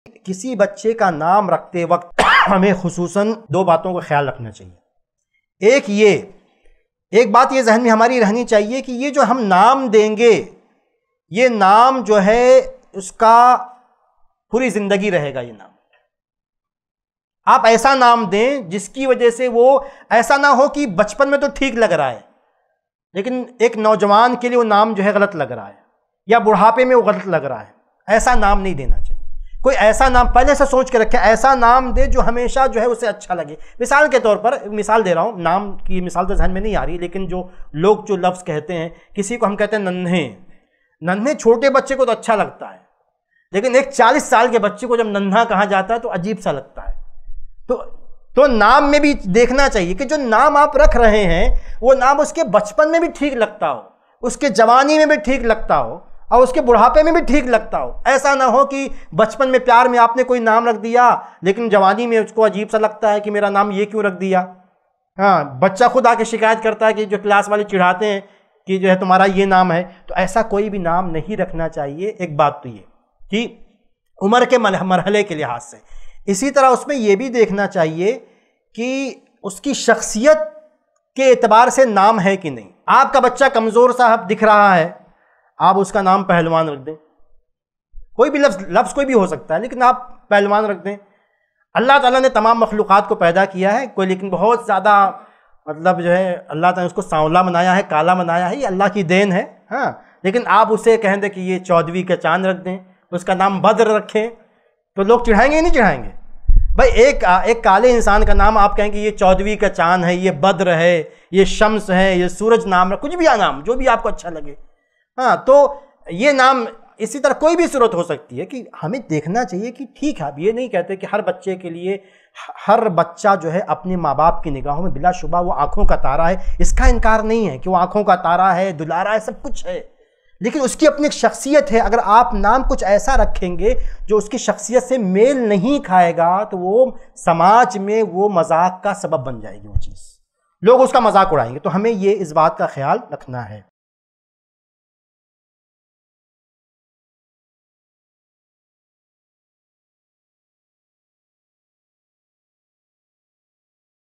Bachchon Ke Naam Rakhte Time In 2 Baaton Ka Dhyaan Rakhein Warna Aayegi Yeh Problem
Bank Account Details:
ILM FOUNDATION
ICICI Bank
Savings account
IFSC CODE-ICIC0001028

Bachchon Ke Naam Rakhte Time In 2 Baaton Ka Dhyaan Rakhein   Warna Aayegi Yeh Problem By Shaikh Abdus Shakoor Madani